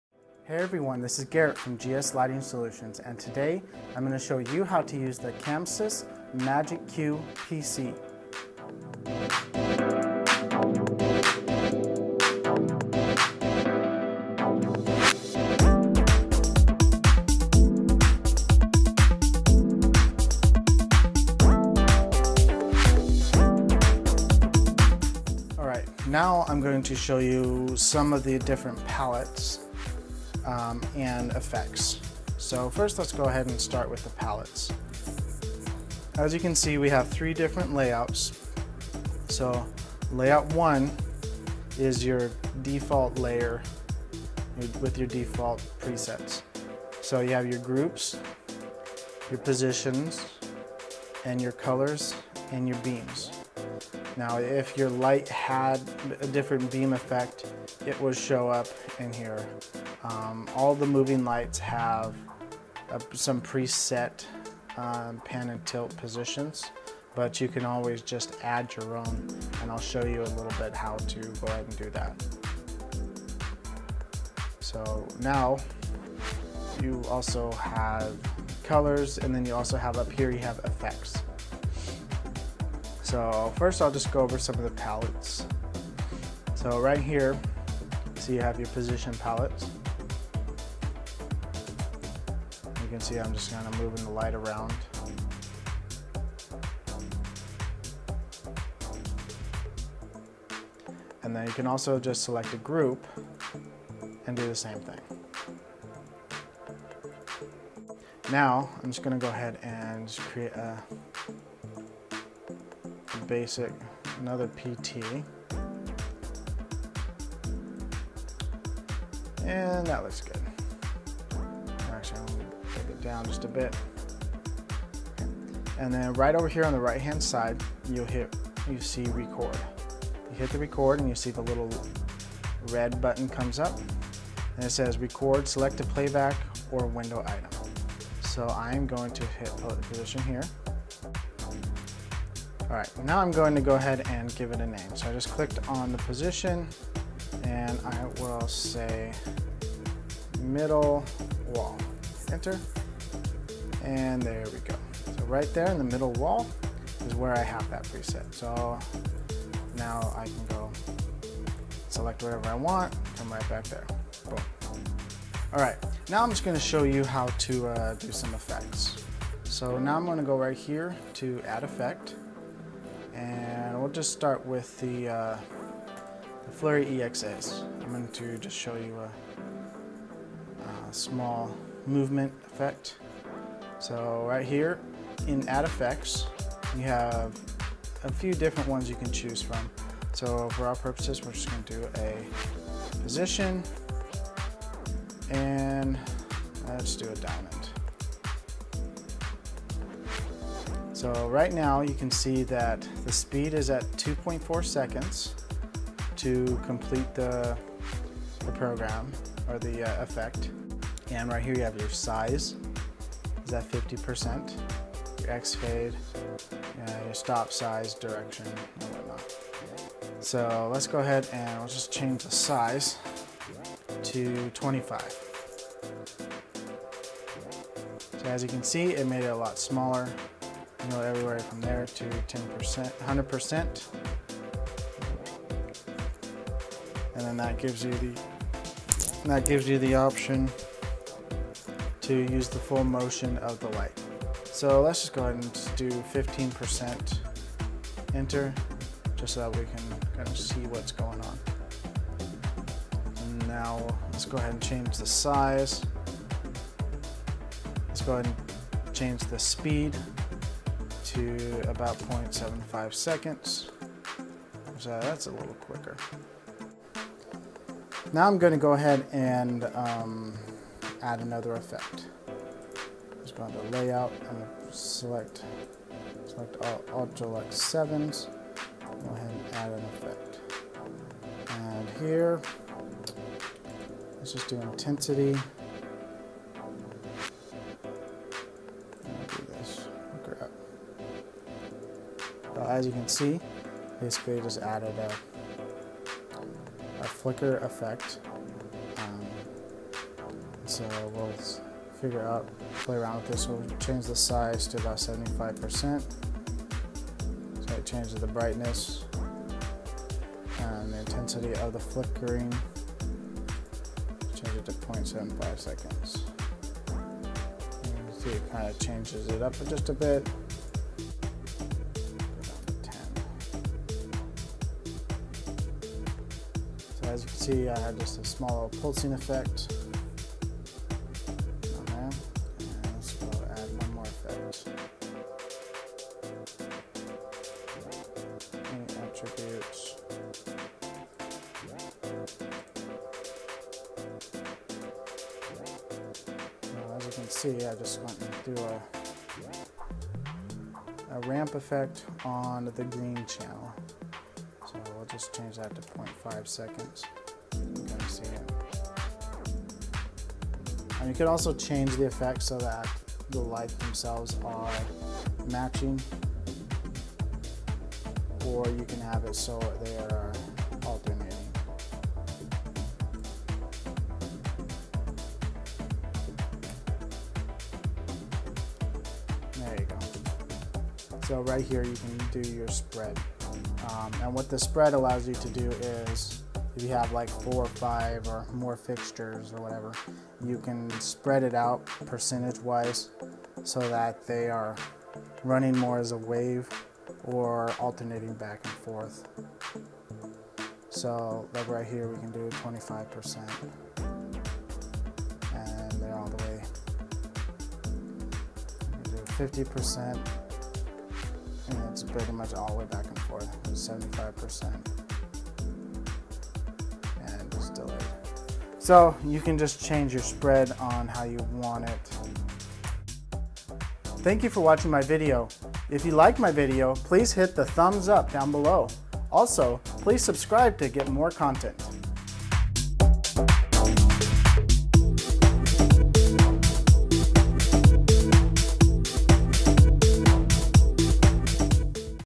Chamsys MagicQ PC Palettes & Effects | Tutorial (2)
In Part 2 of this series I go through some basics of using Palates and effects for building great looks for you venue.

Check out all that you can do with the MagicQ system